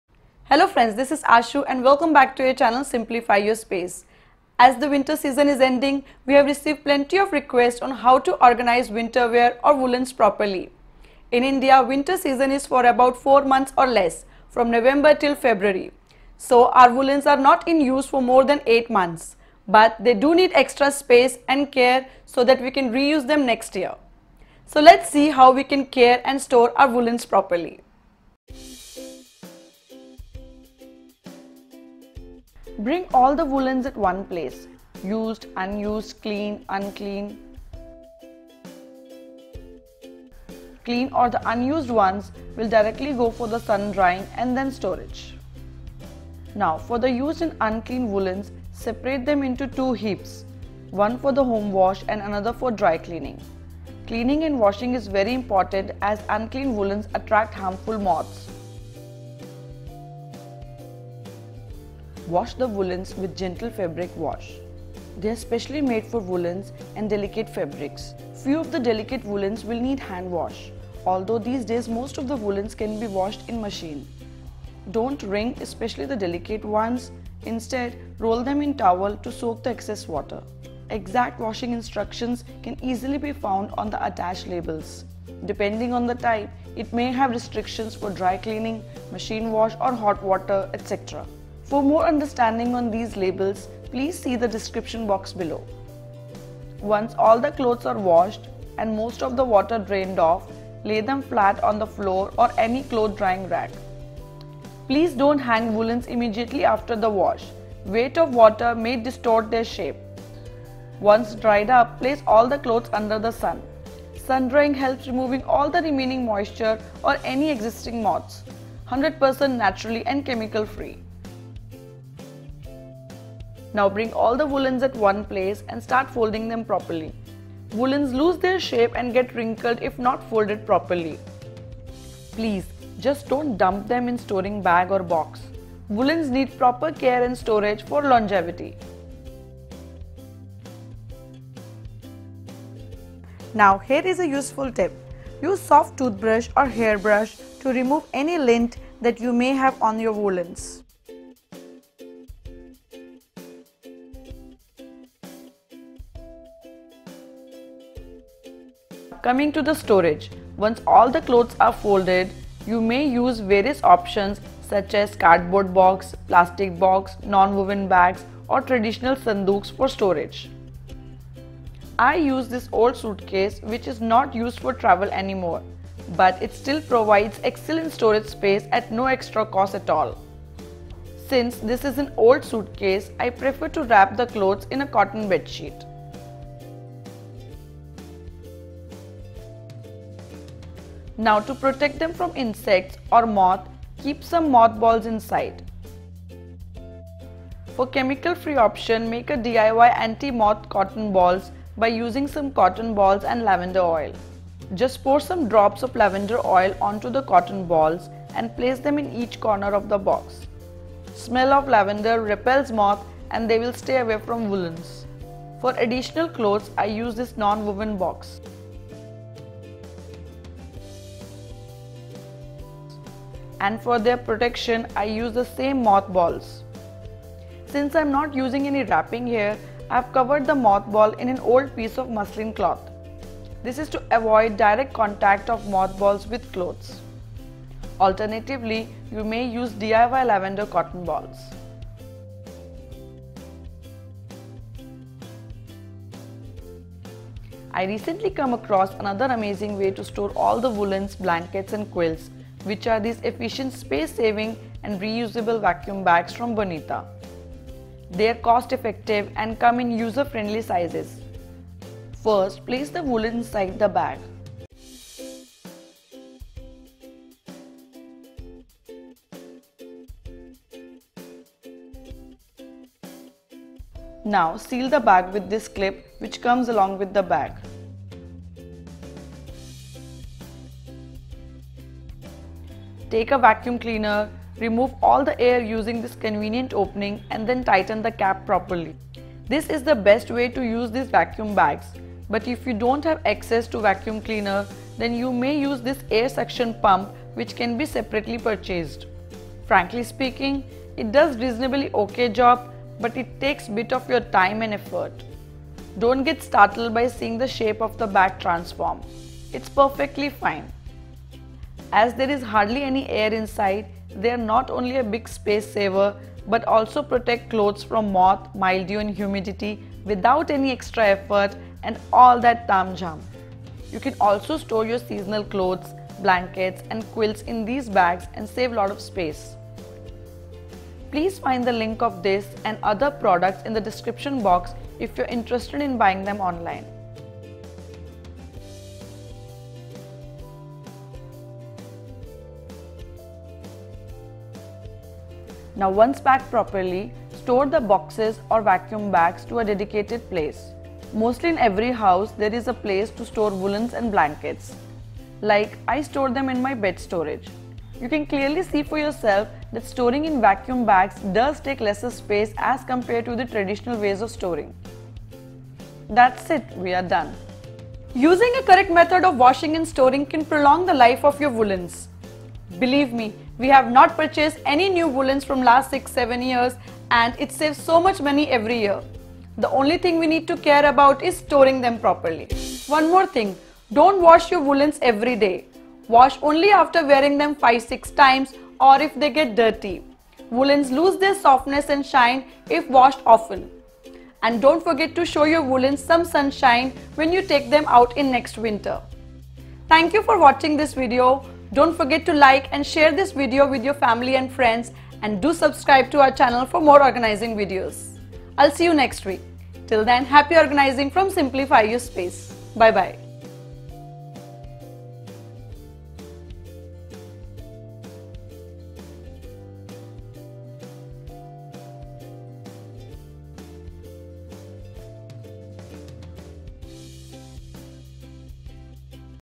How To Care And Store Woollens | Vacuum Storage Bags Guide
This video is on how to care and store the woollens/winterwear properly in 4 simple steps. Additionally, the video has step by step guide for using vacuum storage bags.

For our favorite products visit Amazon store front